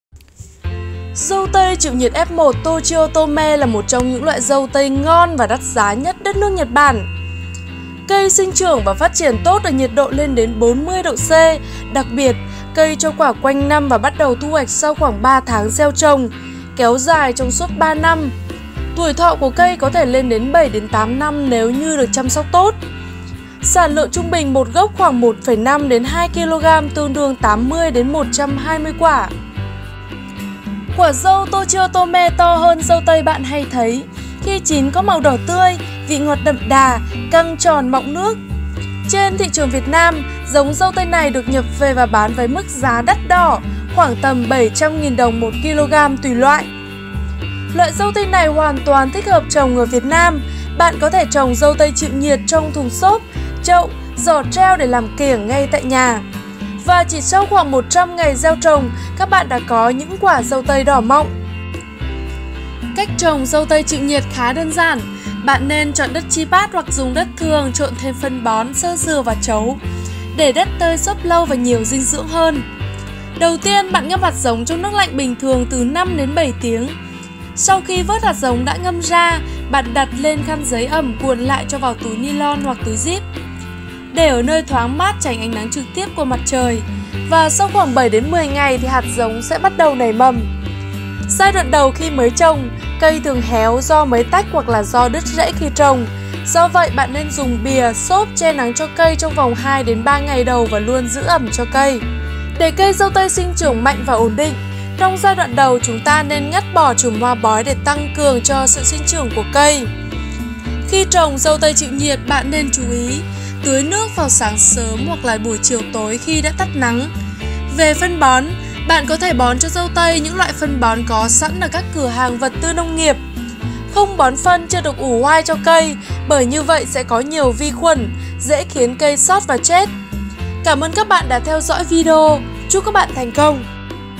Đã mắt với vườn dâu tây Nhật Bản chín mọng
Dâu Tây Chịu Nhiệt F1 Tochiotome là 1 trong những loại dâu ngon và đắt giá nhất đất nước Nhật Bản. Sinh trưởng và phát triển tốt ở nhiệt độ lên tới 40 độ C. Đặc biệt, cây cho quả quanh năm, bắt đầu thu hoạch sau 3 tháng gieo trồng và kéo dài trong suốt 3 năm, tuổi thọ của cây có thể lên đến 7 - 8 năm... Sản lượng trung bình 1 gốc khoảng 1,5 - 2 kg tương đương 80 - 120 quả.
Quả dâu Tochiotome to hơn dâu tây bạn hay thấy, khi chín có màu đỏ tươi, vị ngọt đậm đà, căng tròn mọng nước. Trên thị trường Việt Nam, giống dâu tây này được nhập về và bán với mức giá đắt đỏ khoảng 700 nghìn/kg quả tùy loại.
Loại dâu tây này hoàn toàn thích hợp trồng ở Việt Nam. Bạn có thể trồng dâu tây chịu nhiệt trong thùng xốp, chậu, giỏ treo để làm kiểng ngay tại nhà. Chỉ sau 100 ngày gieo trồng, các bạn đã có những quả dâu tây đỏ mọng.

Cách trồng dâu tây chịu nhiệt khá đơn giản. Bạn nên chọn đất tribat hoặc dùng đất thường trộn thêm phân bón, xơ dừa và trấu để đất tơi xốp lâu, nhiều dinh dưỡng hơn.
Ngâm hạt giống trong nước lạnh bình thường trong 5 – 7 tiếng. Sau khi vớt hạt giống đã ngâm ra, đặt lên khăn giấy ẩm, cuộn lại cho vào túi nilon, để ở nơi thoáng mát, tránh ánh nắng trực tiếp, 7 – 10 ngày sau hạt giống sẽ bắt đầu nảy mầm.
Giai đoạn đầu khi mới trồng, cây thường héo do mới tách hoặc do đứt rễ khi trồng, do vậy bạn nên dùng bìa, xốp,… che nắng cho cây trong vòng 2-3 ngày đầu và phải luôn giữ ẩm.
Để cây dâu tây sinh trưởng mạnh và ổn định, trong giai đoạn đầu chúng ta nên ngắt bỏ chùm hoa bói để tăng cường cho sự sinh trưởng của cây.
Khi trồng dâu tây chịu nhiệt bạn nên chú ý tưới nước vào sáng sớm hoặc buổi chiều khi đã tắt nắng. Về phân bón, bạn có thể bón cho dâu tây những loại phân có bán sẵn tại các cửa hàng vật tư nông nghiệp. Không bón phân chưa được ủ hoai cho cây, bởi có nhiều vi khuẩn, dễ khiến cây xót và chết.
Cảm ơn các bạn đã theo dõi video. Chúc các bạn thành công!